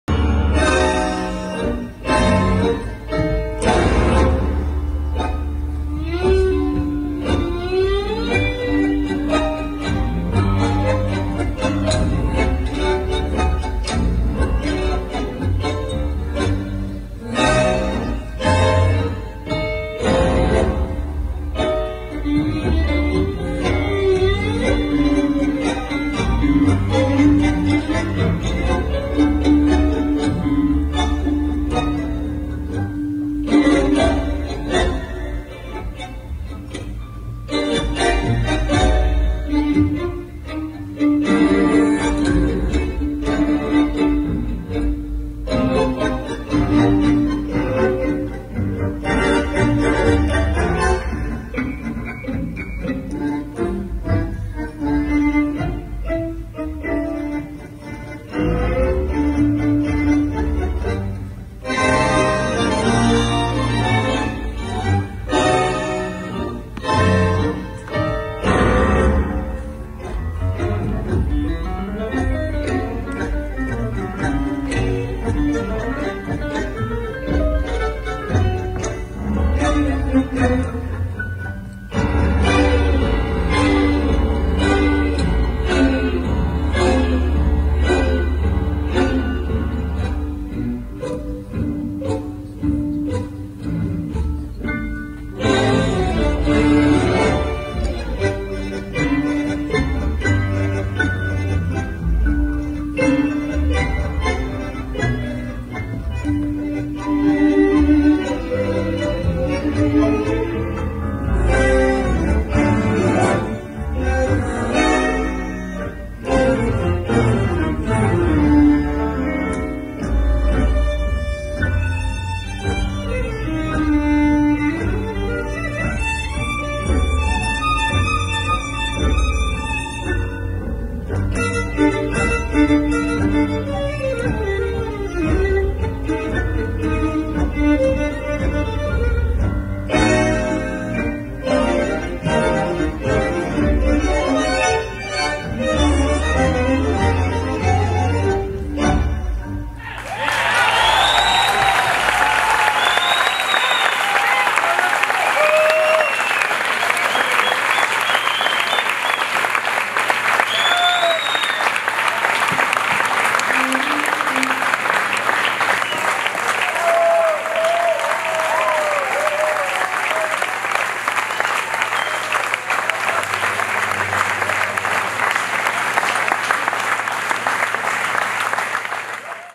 Rodrigo Rufino & Gisela Passi - La Viruta avec l'Orquesta Silbando - Tango Roots Festival
Tango Roots Festival
Rodrigo Rufino & Gisela Passi
La Viruta avec l'Orquesta Silbando

Images A. Alejandro Rumolino - Créations